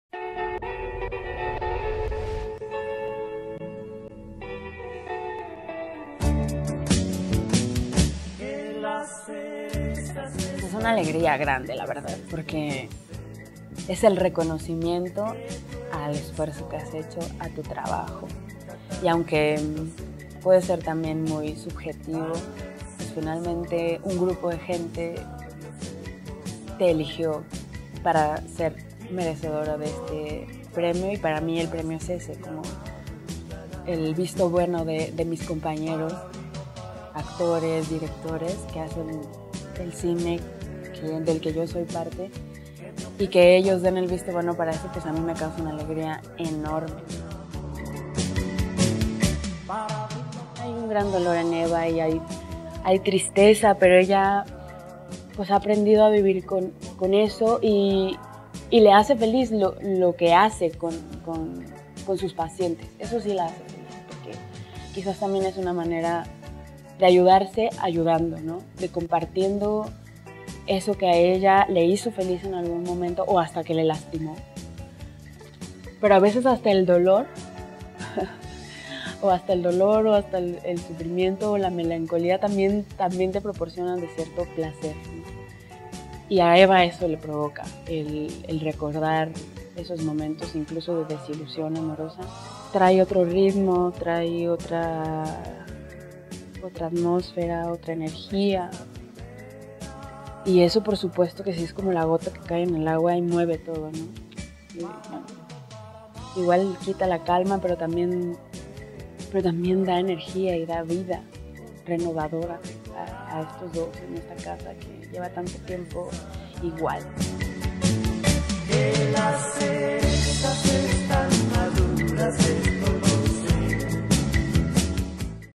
Entrevista con Adriana Paz. Parte 2 "La Caridad"
Después de ganar dos Arieles, Adriana Paz sigue con la misma humildad y dedicación hacia su profesión, sin duda una mujer que vive y respira la actuación. En esta entrega también le preguntamos sobre Eva, su personaje en LaCaridadFilm, esto fue lo que nos dijo...

¿Quiéres saber más sobre esta película?